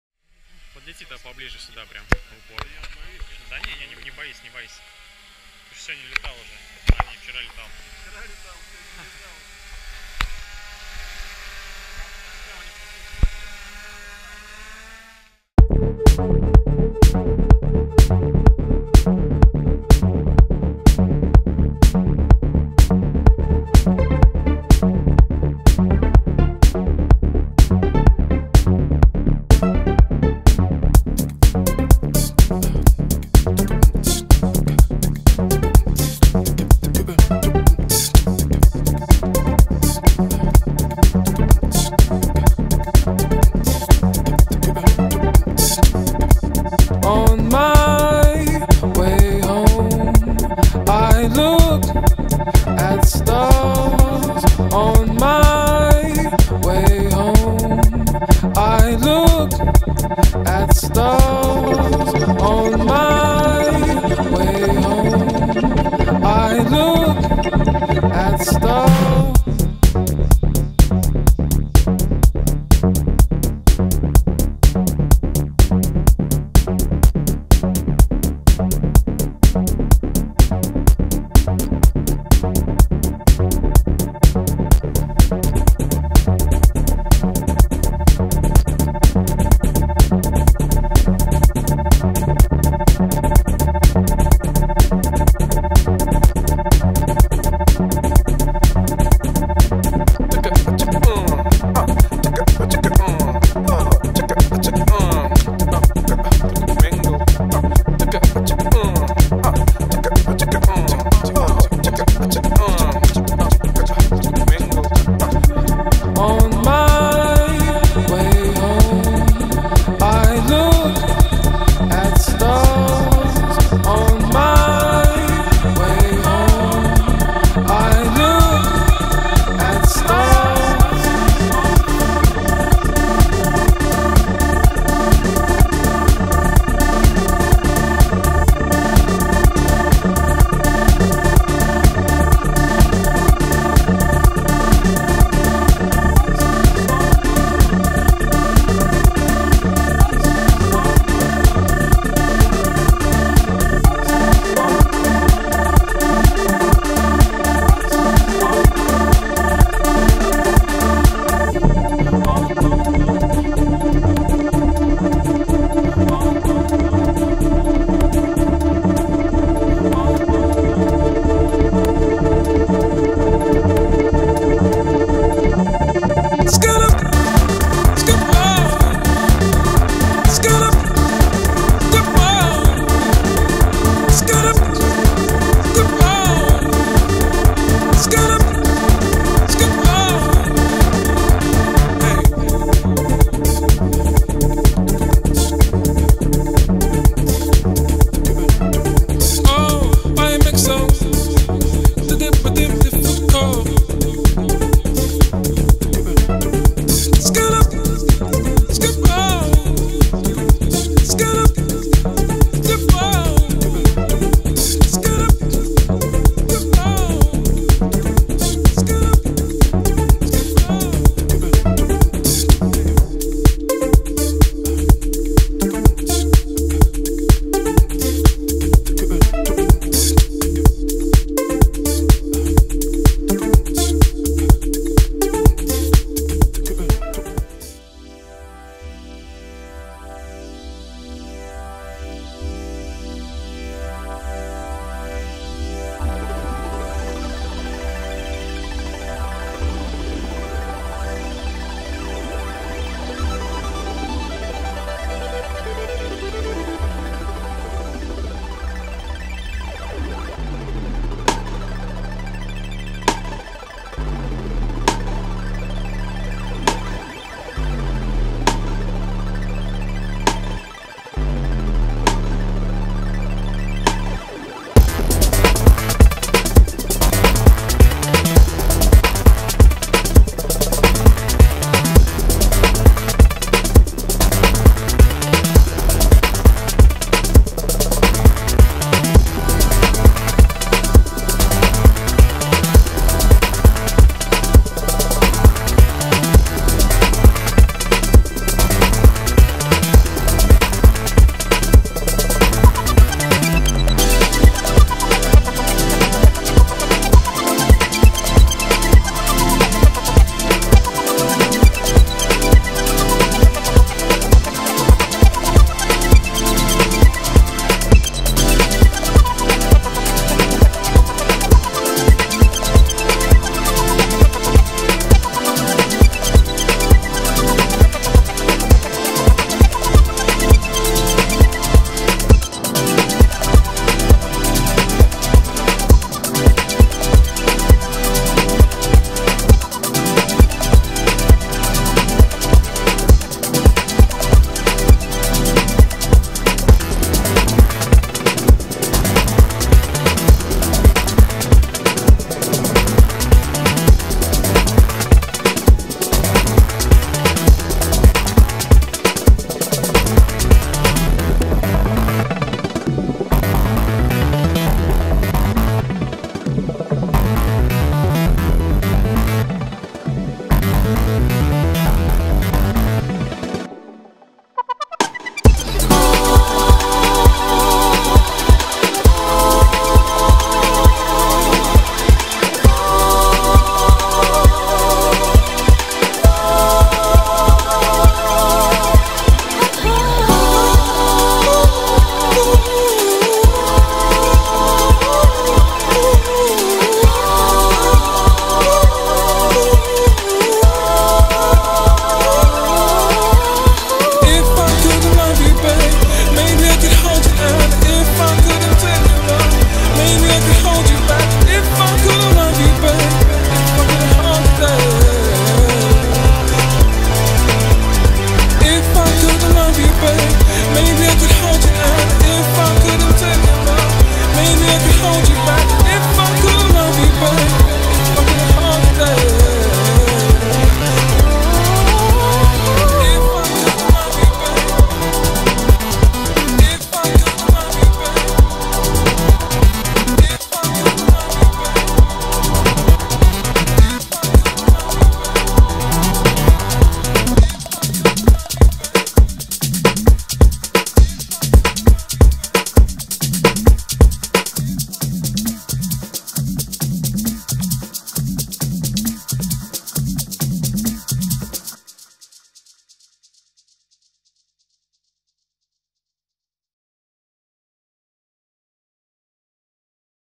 Гексакоптер. Летные испытания. Фронтальная камера.
3-й день моей летной практики на самодельном гексакоптере. Записано на Xiaomi Yi. В полетном контроллере установил ограничение по радиусу и высоте полета в 15 метров, так что особо не разлетался, да и народу кругом было много, безумные детишки - опасно.

В ролике использованы композиции SBTRKT.